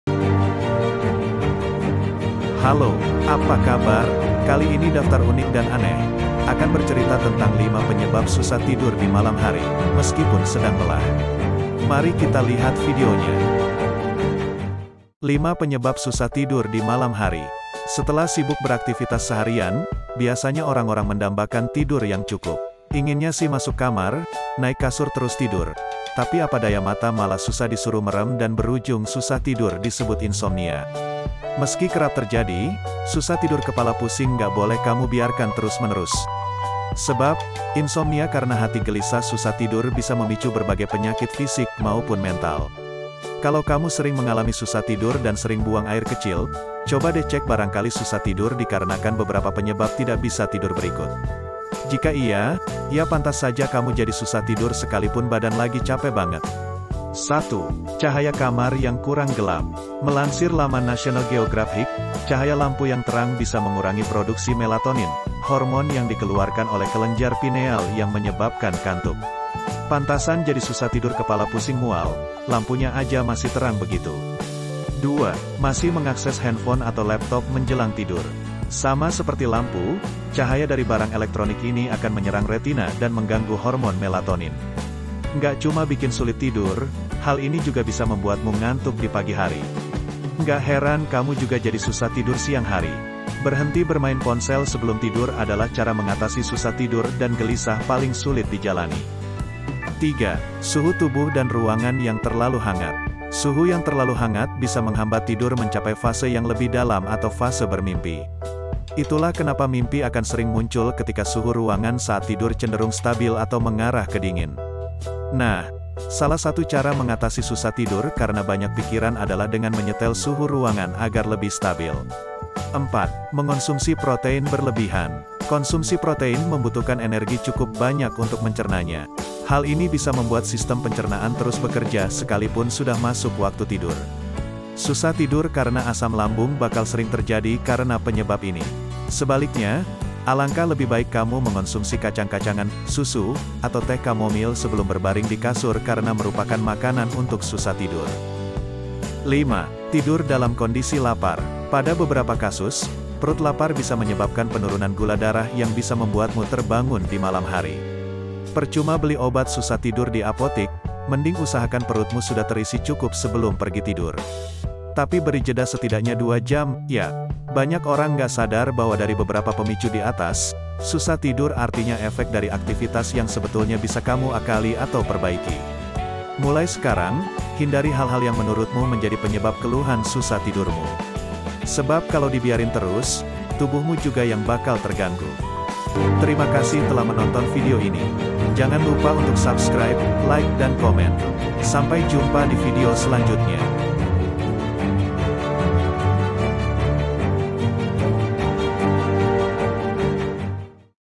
Solusi Unik Dan Aneh Mengatasi 5 Penyebab Susah Tidur Di Malam Hari
Kalau kamu sering mengalami susah tidur dan sering buang air kecil, coba deh cek barangkali susah tidur dikarenakan beberapa penyebab tidak bisa tidur berikut. Jika iya, ya pantas saja kamu jadi susah tidur sekalipun badan lagi capek banget.